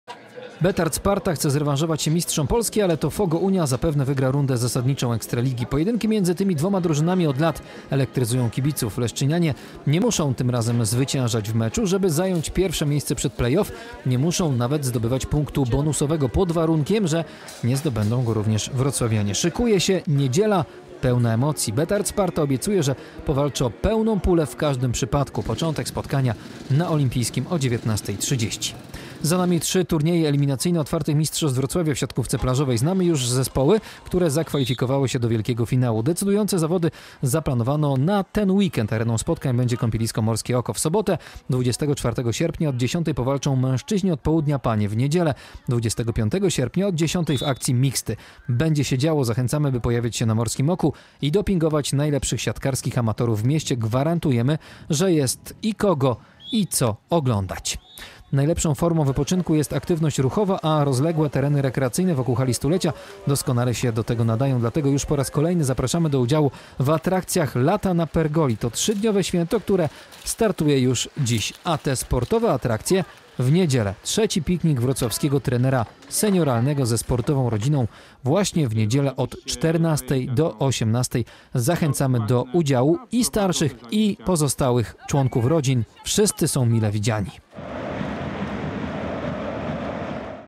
Weekend sportowych atrakcji we Wrocławiu
Najważniejszym sportowym wydarzeniem weekendu będzie mecz żużlowców. W niedzielę na Stadionie Olimpijskim zostanie rozegrany największy hit czternastej kolejki rozgrywek PGE Ekstraligi. Betard Sparta Wrocław podejmować będzie lidera tabeli Fogo Unię Leszno. Poza tym we Wrocławiu w weekend finały OMW w siatkówce plażowej i festyn Lato z Trenerem Osiedlowym.